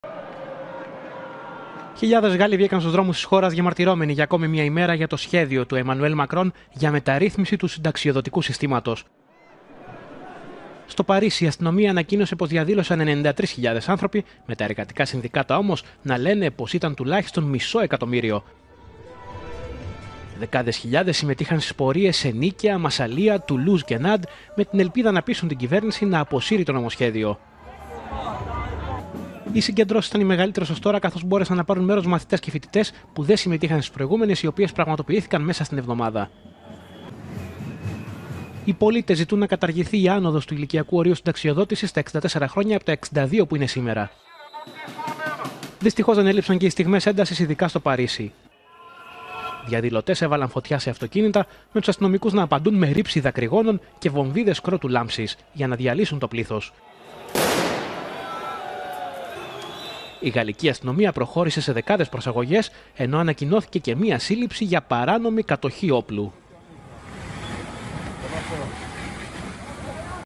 Γαλλία: Μαζική διαδήλωση κατά του Μακρόν - Επεισόδια στο Παρίσι
Οι πολίτες διαμαρτύρονται για την αύξηση του ορίου συνταξιοδότητης στα 64 χρόνια